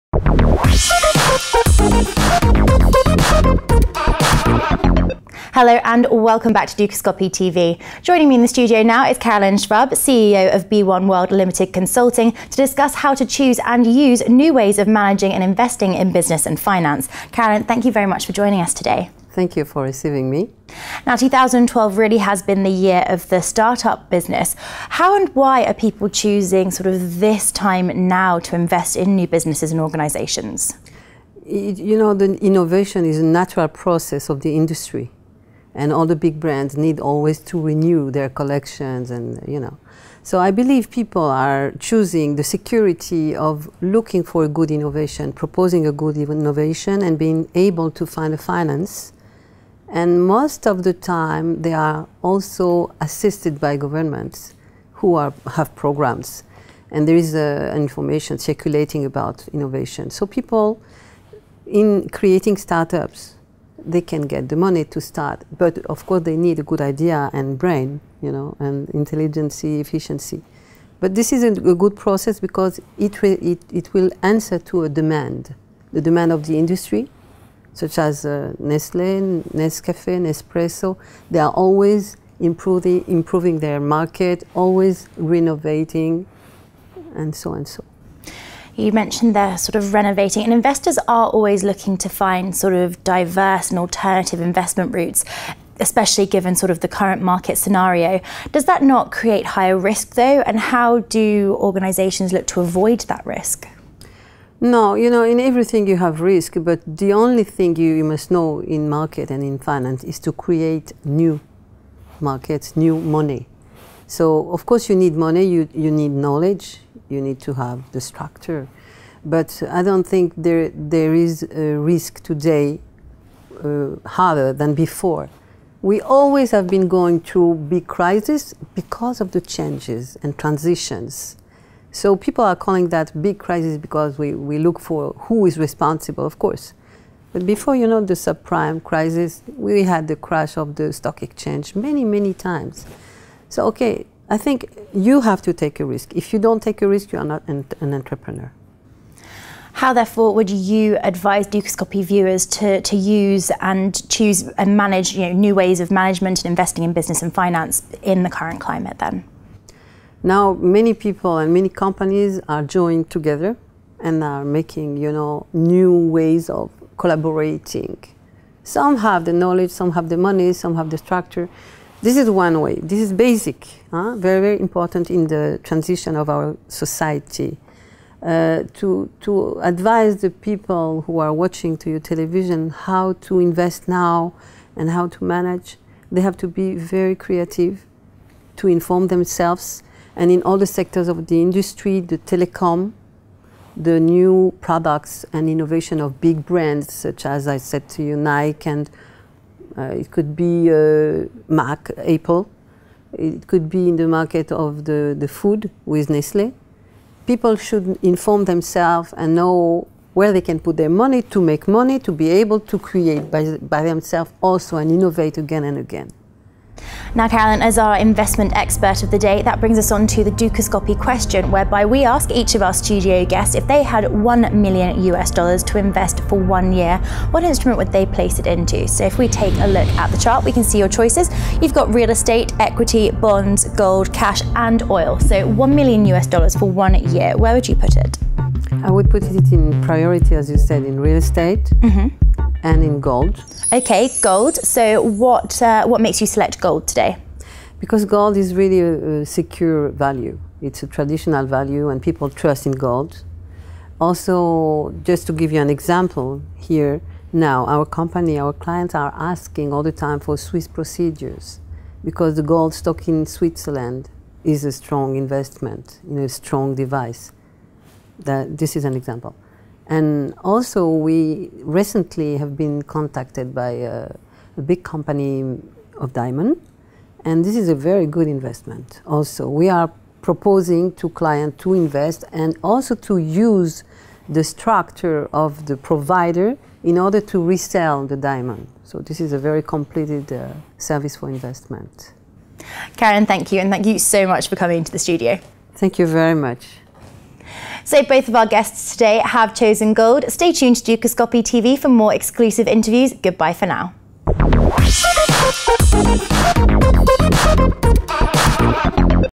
Be1World on Business
Caroline Schwab, Founder and CEO of Be1World Ltd, joins Natalie MacDonald in the studio to discuss how to choose and use new ways of managing and investing in business and finance.